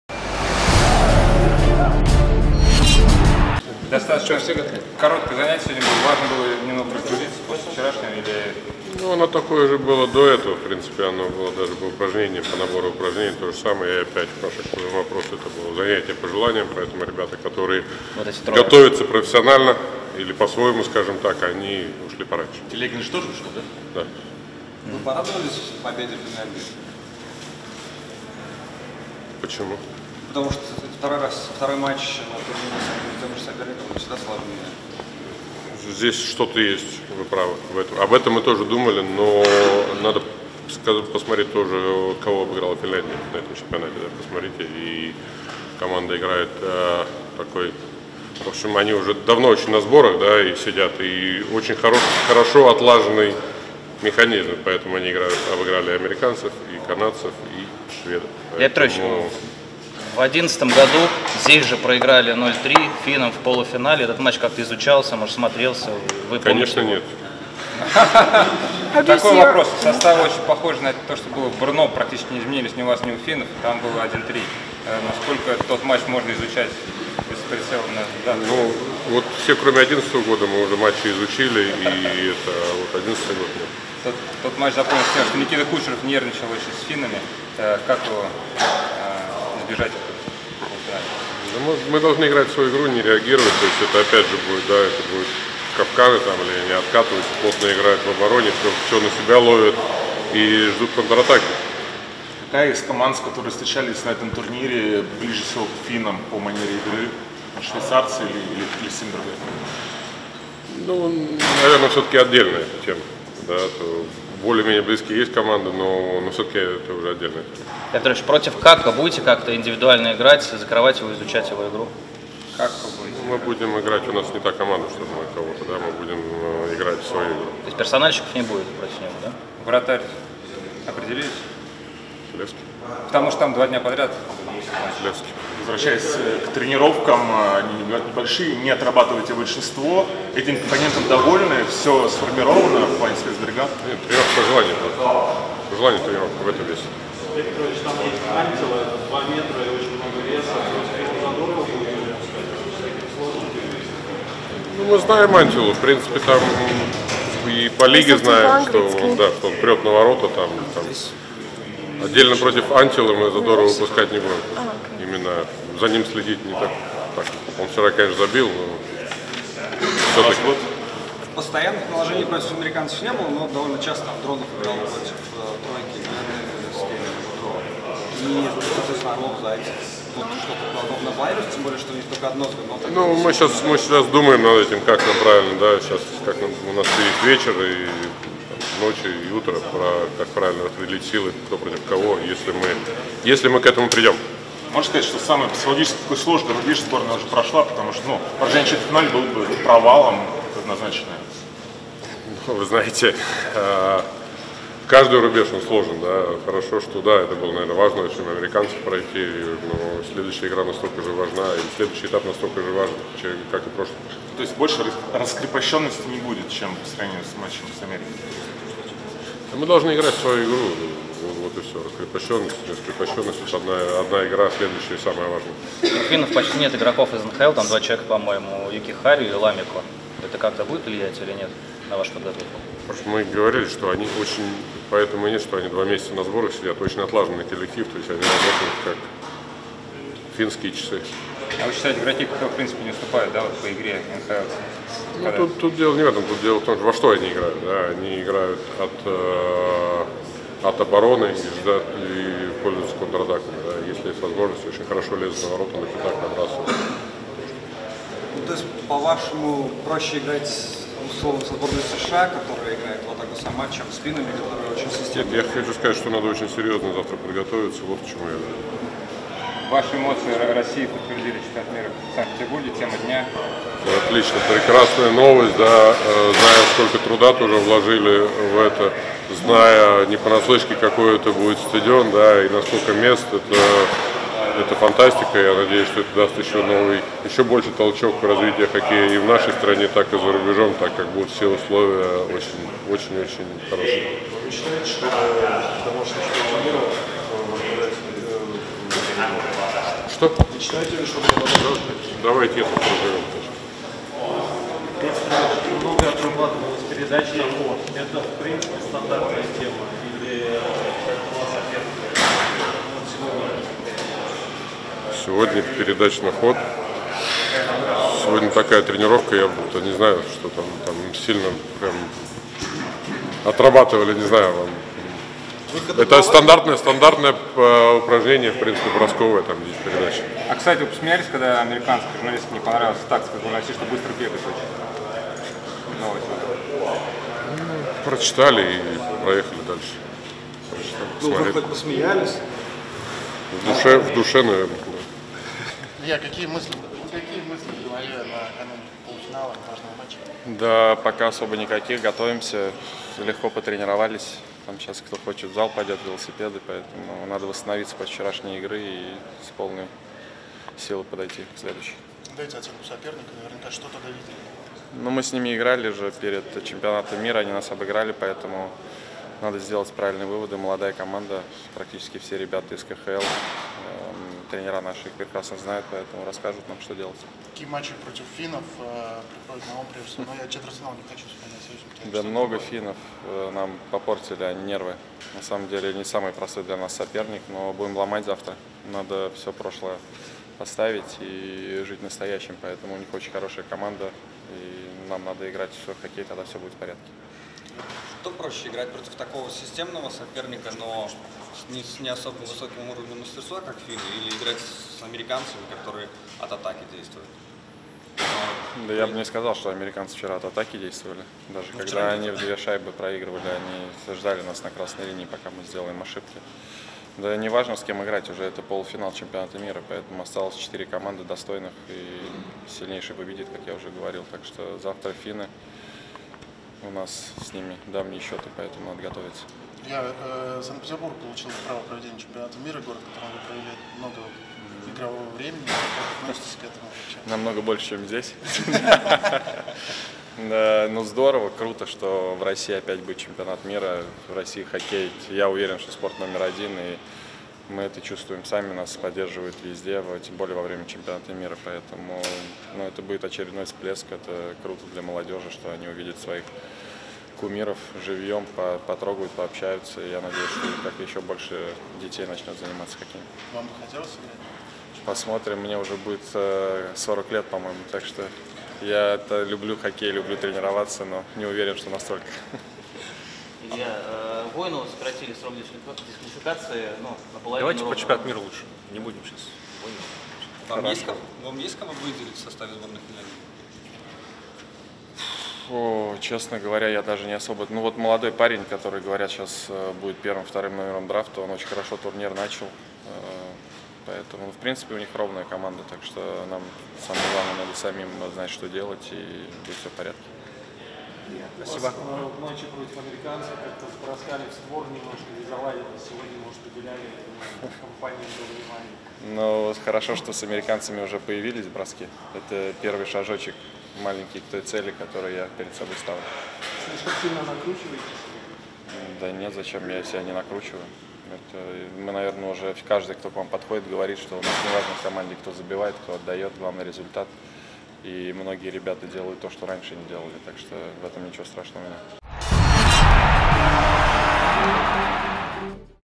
"Почему мы должны радоваться победе финнов?". Воробьев и Ковальчук перед полуфиналом ЧМ
Слова главного тренера и капитана сборной России перед матчем с Финляндией